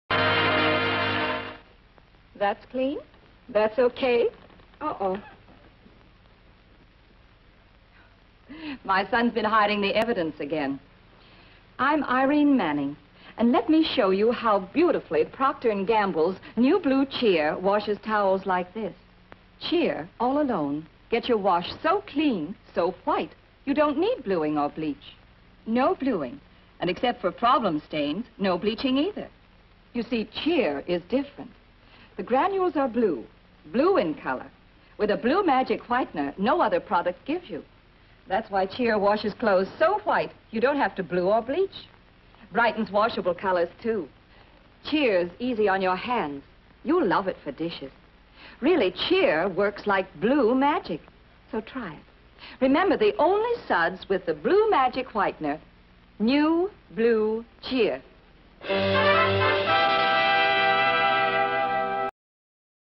Classic Television Commercial for ''Blue Cheer''
Another classic television commercial for the cleaning product "Blue Cheer". This product was marketed for use on both clothes and dishes. I don't know the exact date, though it appears to be from the early 1950's, circa december